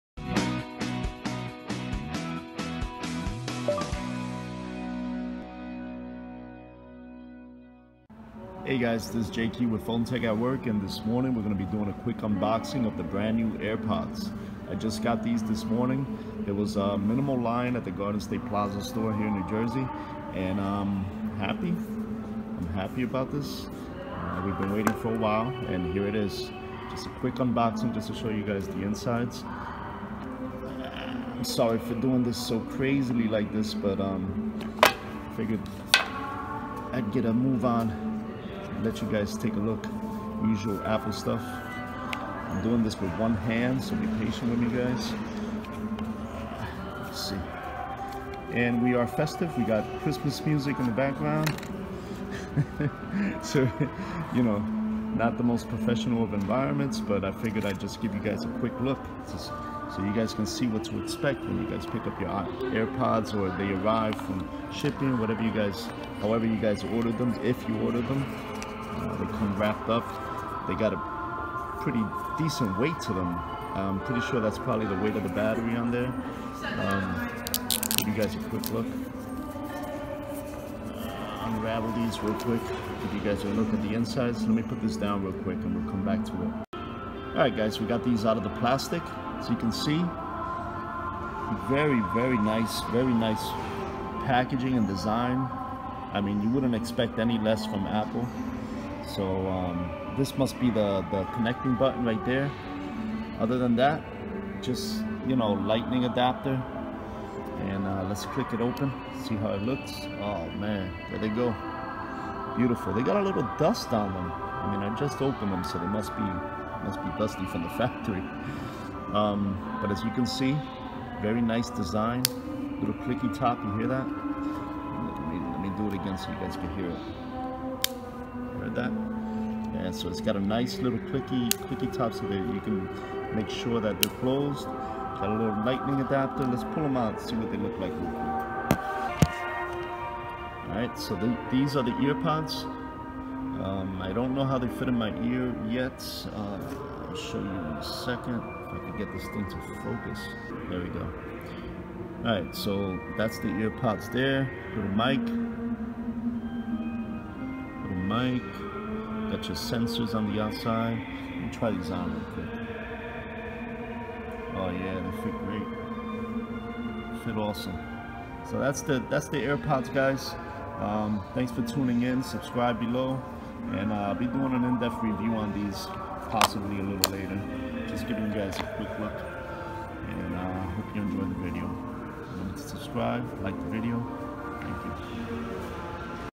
Apple Airpods Unboxing Holiday Edition
This was filmed the day the Apple Store released them. Late upload